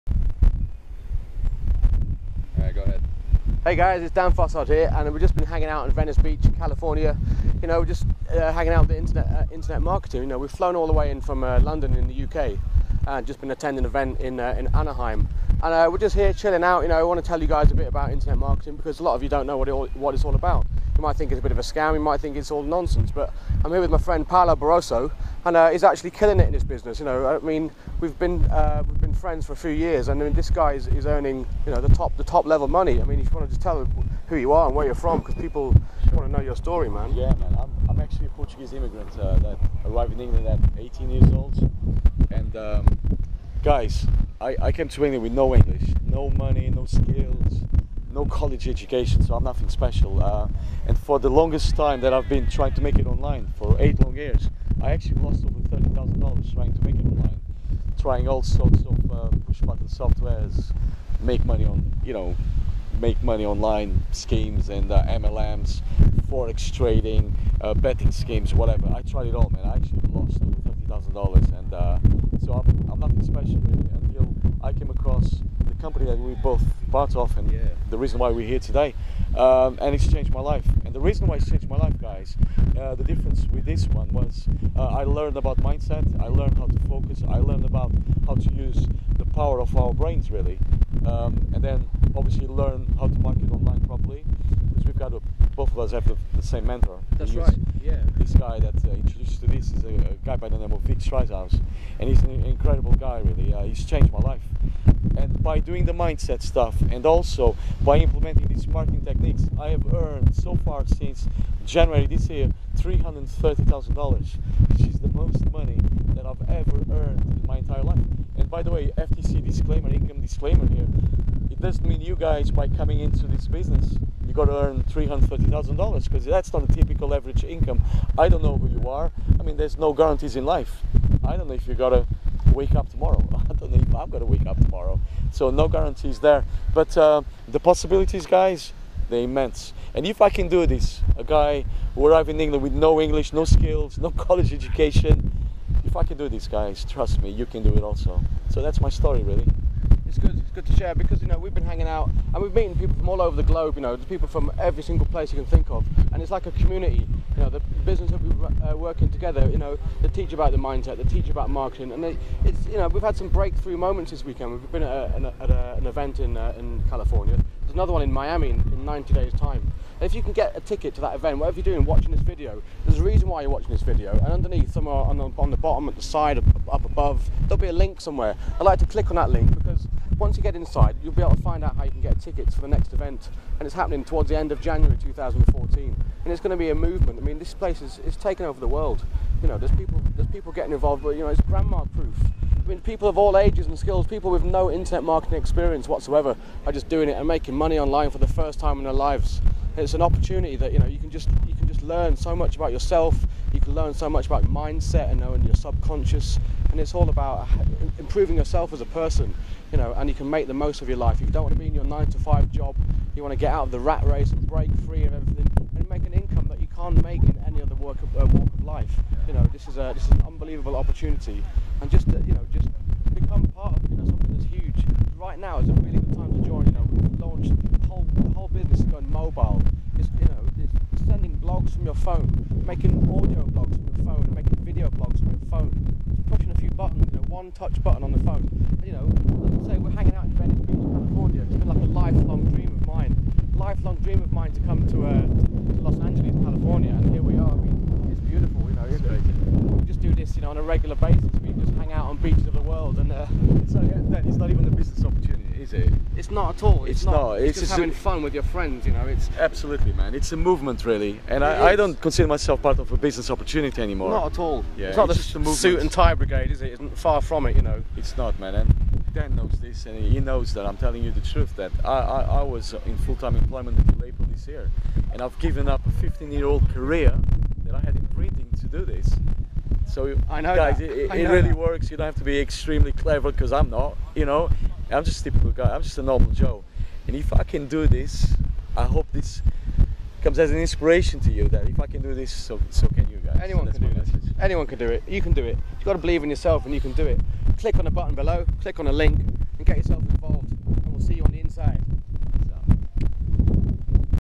Paulo Barroso Speaking On Venice Beach, California
(ENTER YOUR EMAIL HERE) (JOIN HERE)
Check out this brand new interview with my good personal friend Mr Paulo Barrroso.
Click the link & we'll see you on the beaches of the world.